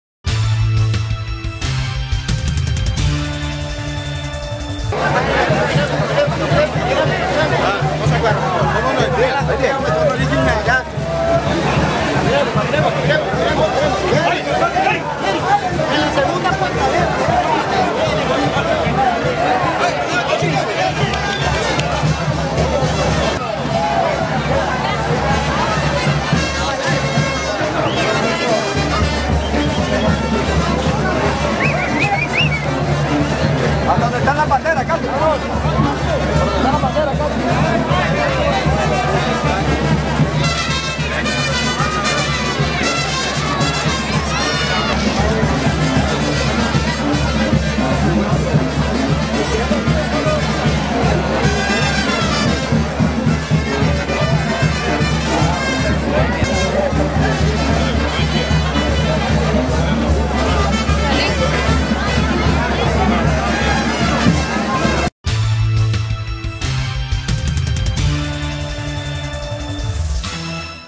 La Octava de "Tata Chombo"
Miles de devotos de San Jerónimo, el santo milagroso de los masayas, iniciaron este lunes la tradicional Octava, una procesión que recorre por más de 24 horas continuas la ciudad de Masaya.